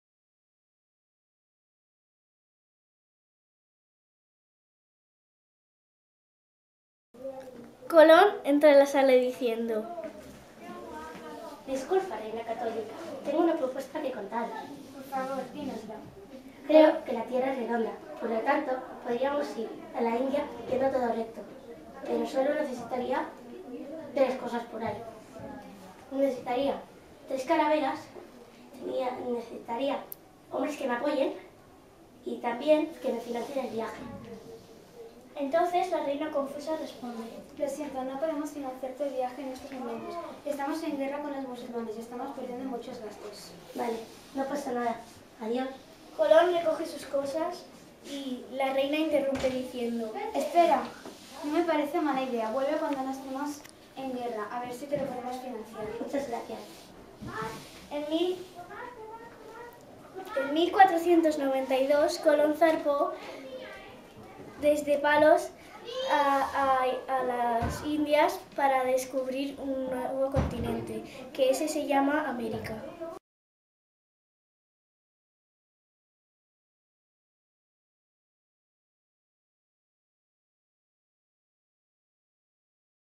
La reunión con la reina Carla Uri Aina
Teatre dels alumnes de 6è de l'Escola Les Fonts d'Argentona sobre l'audiència que va tenir en Cristòfol Colom abans d'arribar a les Amèriques.